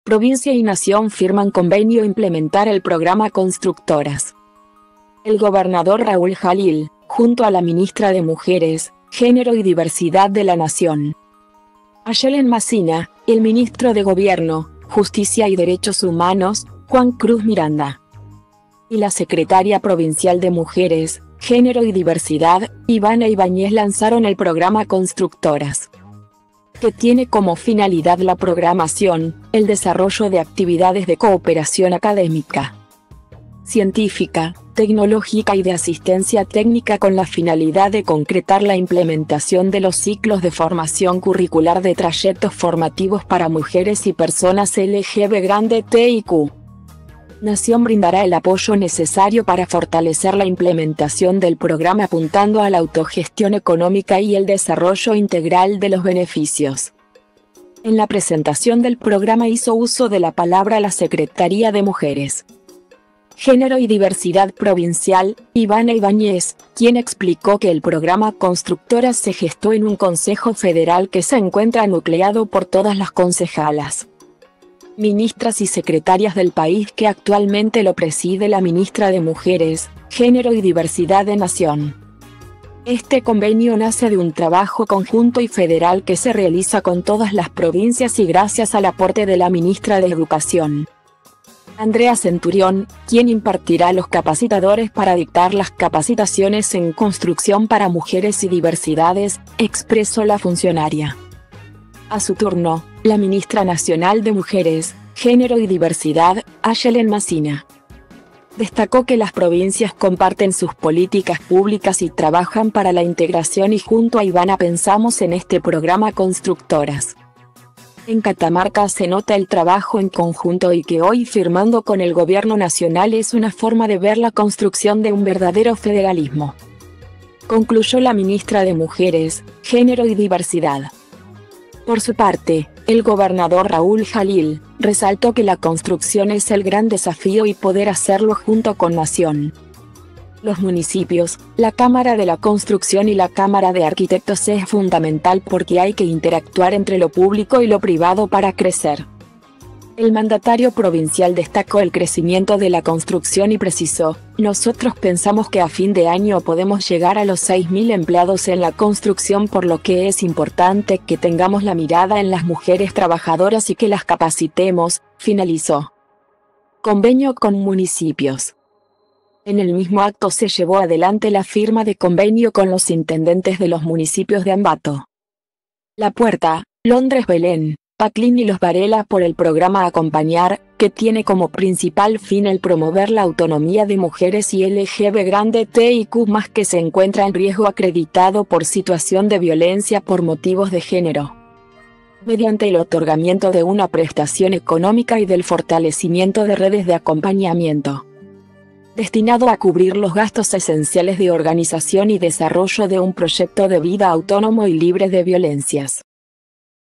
Provincia y Nación firman convenio implementar el programa Constructoras
El gobernador Raúl Jalil, junto a la ministra de Mujeres, Género y Diversidad de la Nación,Ayelen Mazzina, el ministro de Gobierno, Justicia y Derechos Humanos, Juan Cruz Miranda,y la Secretaria provincial de Mujeres, Género y Diversidad, Ivana Ibañez lanzaron el programa "Constructoras",